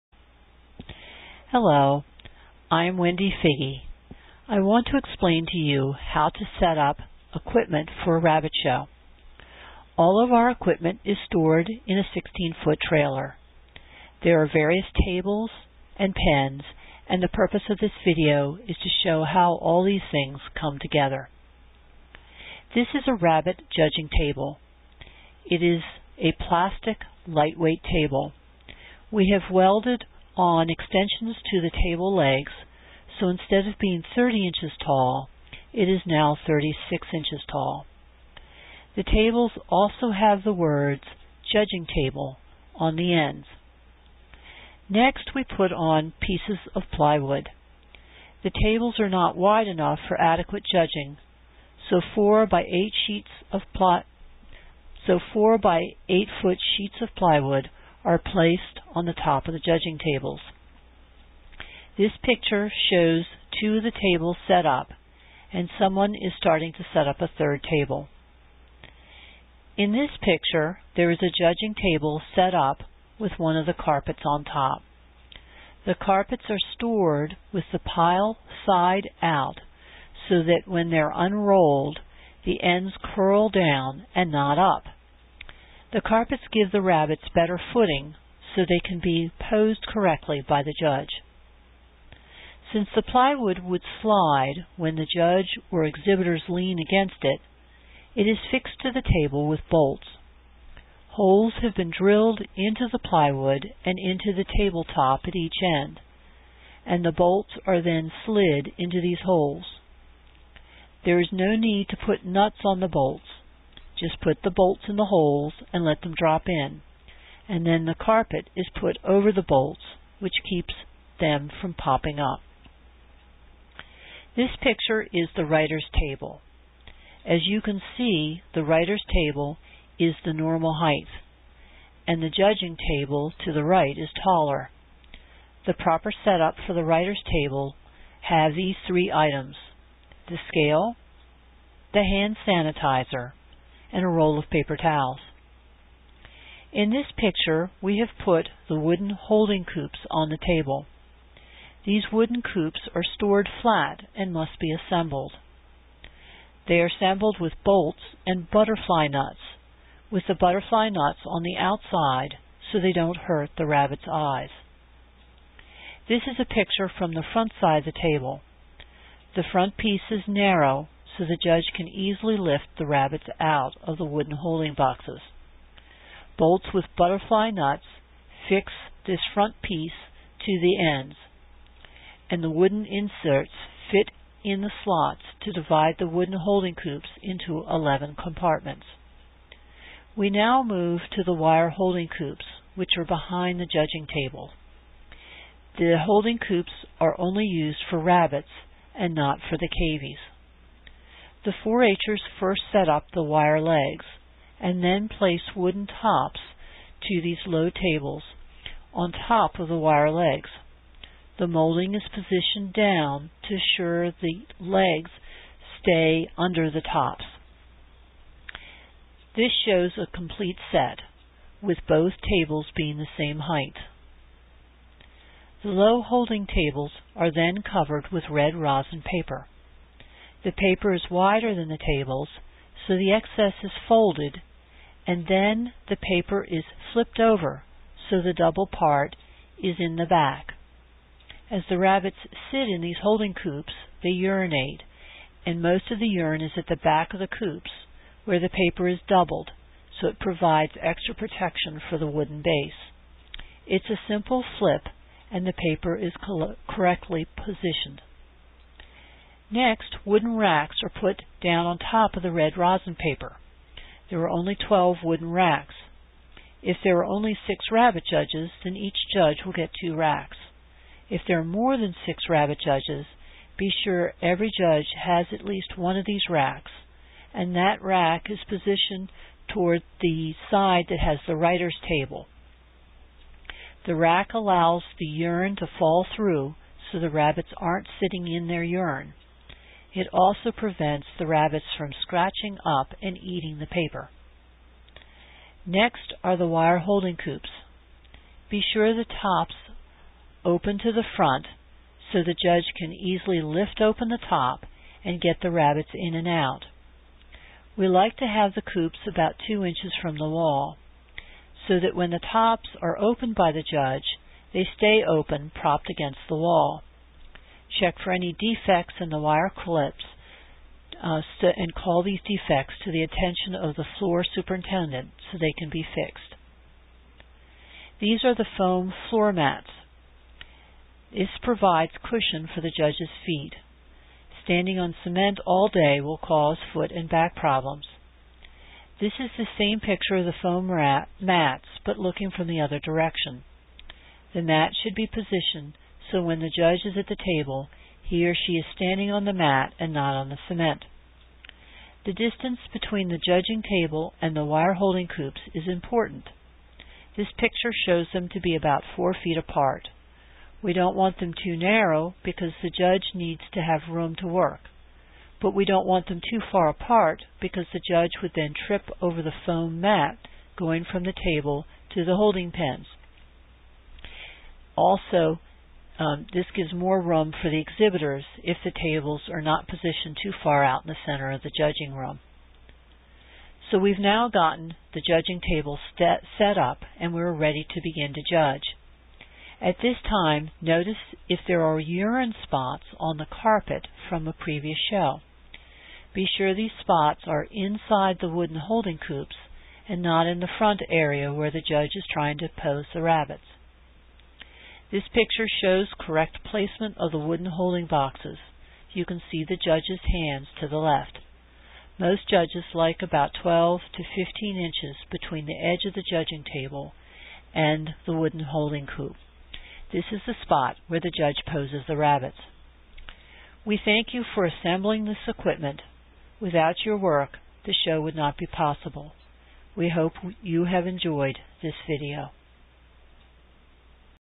Rabbit Show Set up2
How to set up rabbit show equipment for the Baltimore & Howard Rabbit Club.